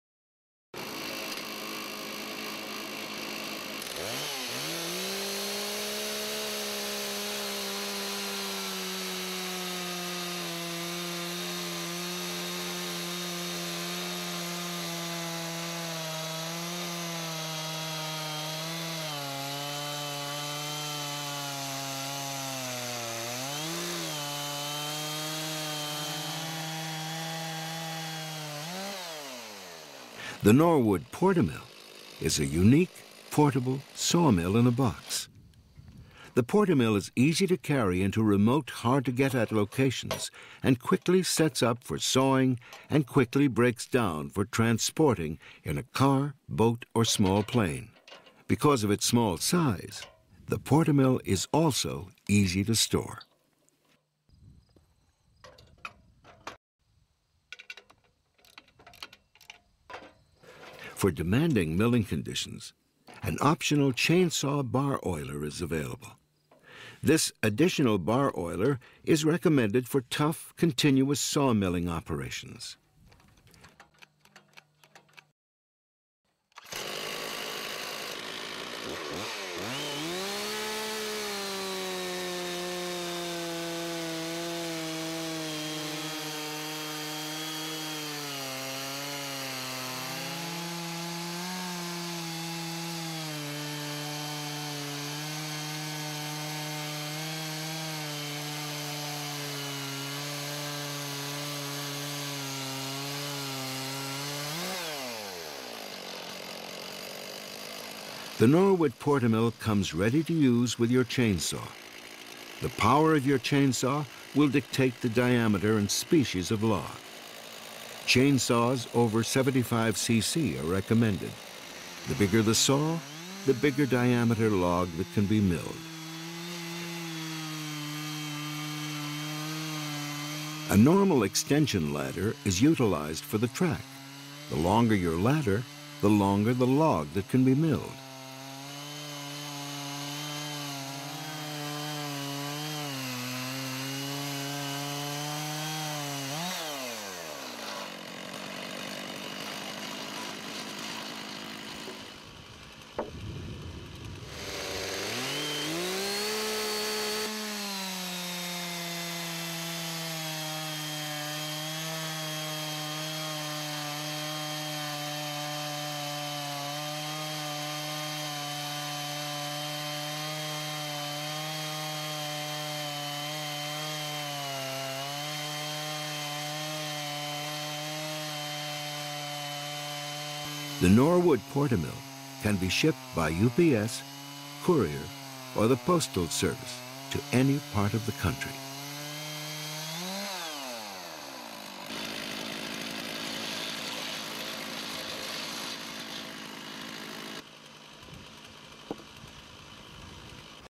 Norwood PortaMill Chainsaw Sawmill - Make Your Own Lumber
With the Norwood PortaMill, your chain saw and your household ladder, you'll start making accurate, smooth dimensional lumber, from 2 x 4s to 6 x 6s. Handles green logs up to 14in. dia. and as long as your ladder permits. Your chain saw is the ...